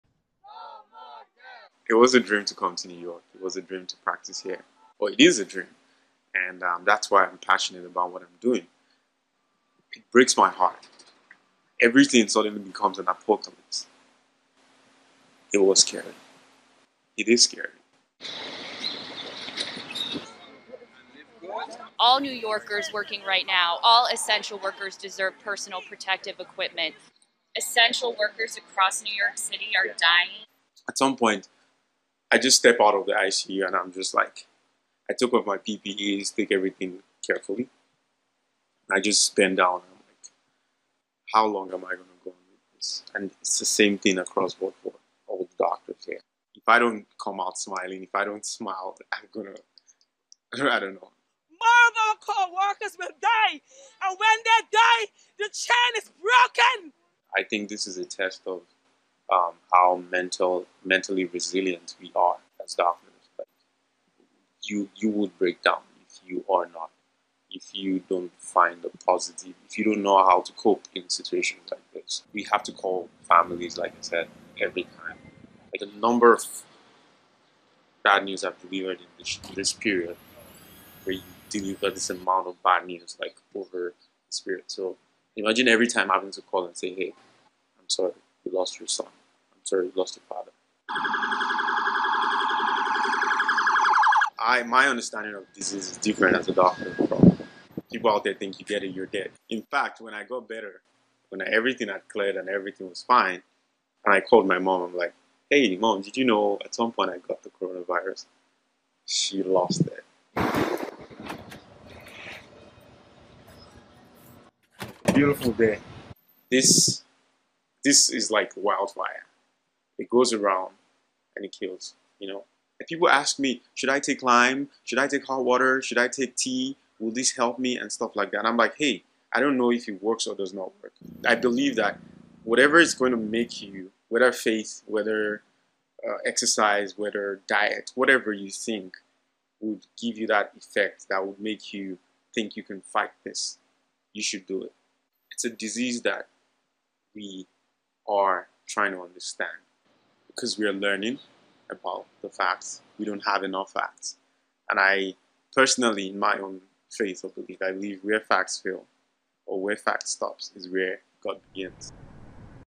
Recovered doctor shares thoughts about situation in New York
New York has been the epicenter of America's fight against COVID-19. A Nigerian doctor based there has recovered from coronavirus and talks to CGTN about his experience, as well as the situation in the U.S. state.